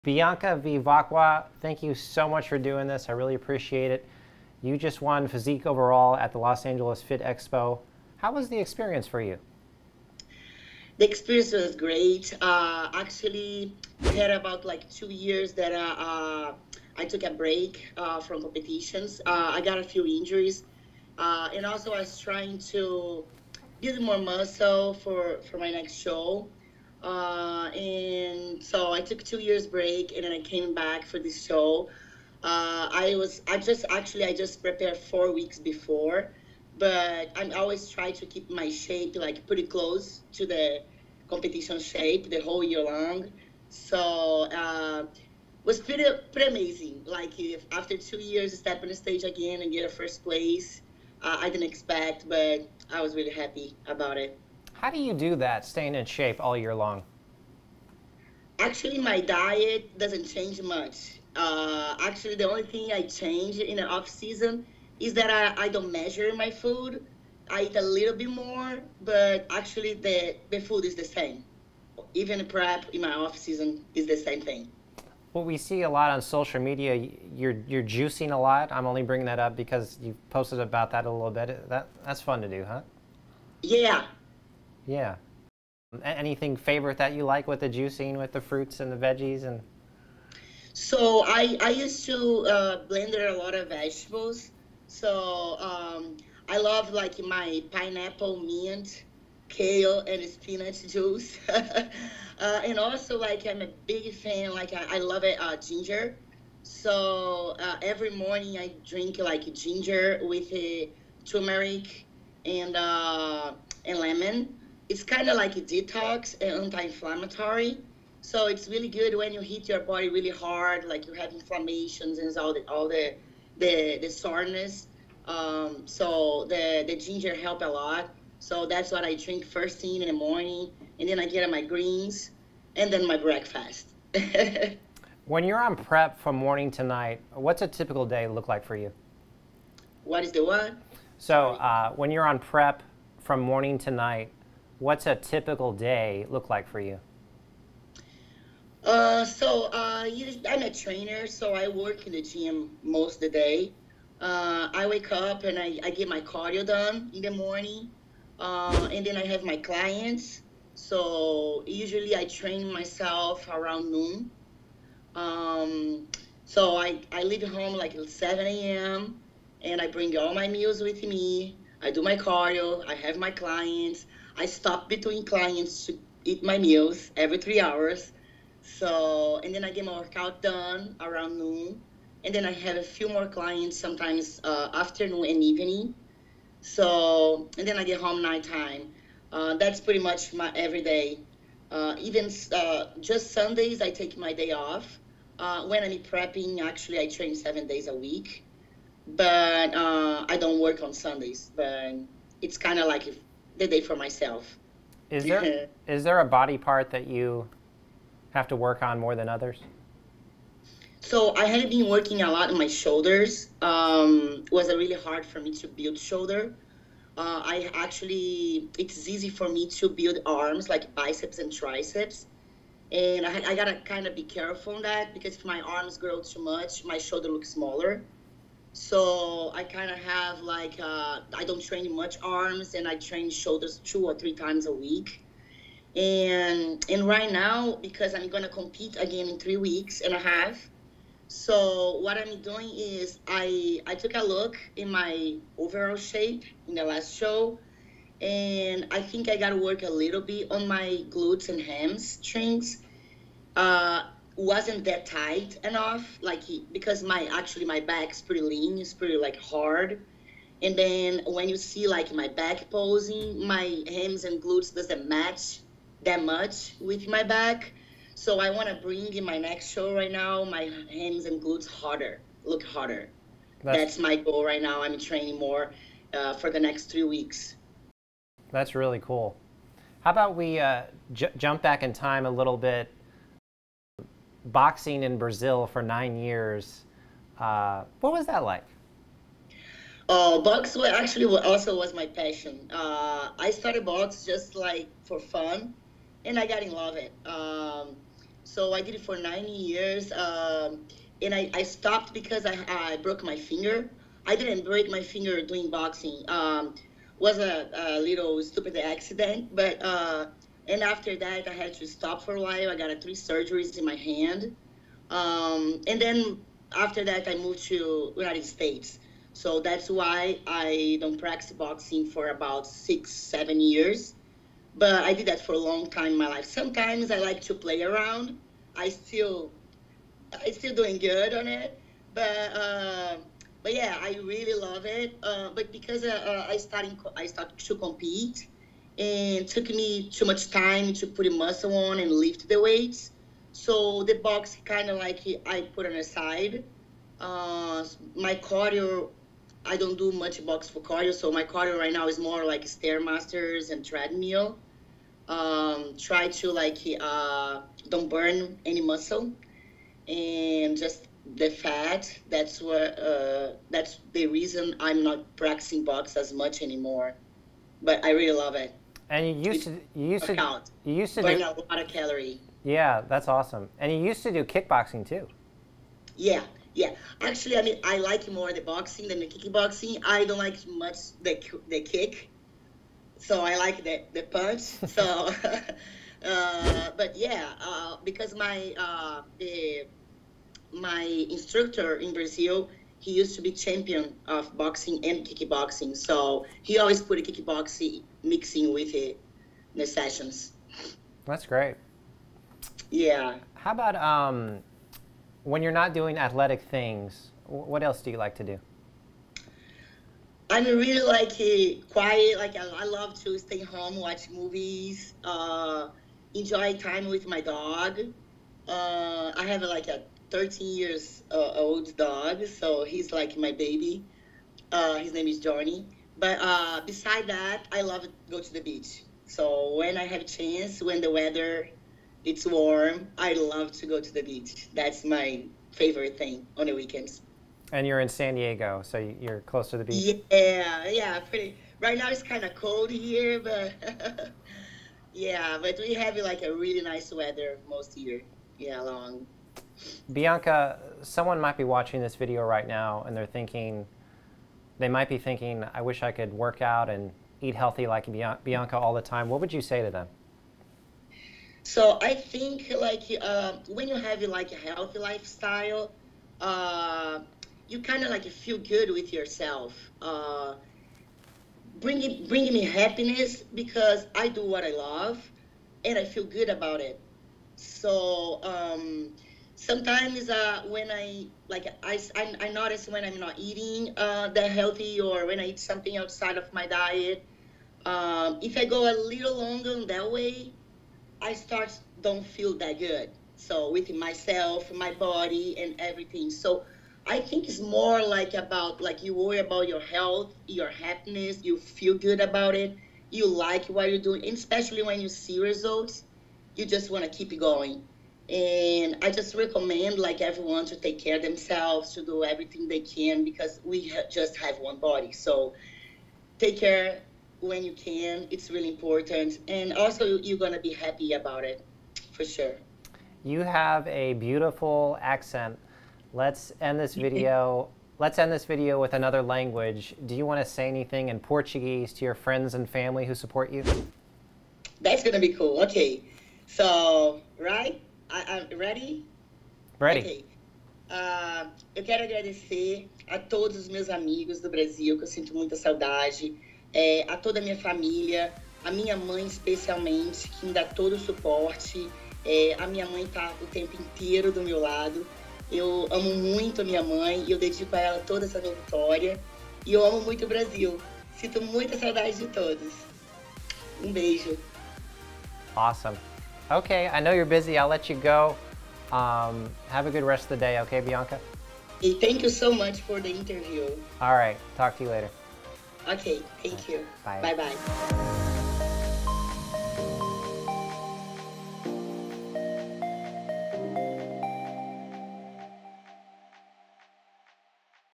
LIVE with Overall Physique Champion Bianca Vivaqua⭐️
There are no annoying pre-roll ads.

LA Fit Expo Women's Physique Overall Champion Bianca Vivaqua was kind enough to let me interview her live for you guys to watch. She is originally from Brazil. Enjoy!

Chris Ghanbari
Director of Video Services at Real News PR, LLC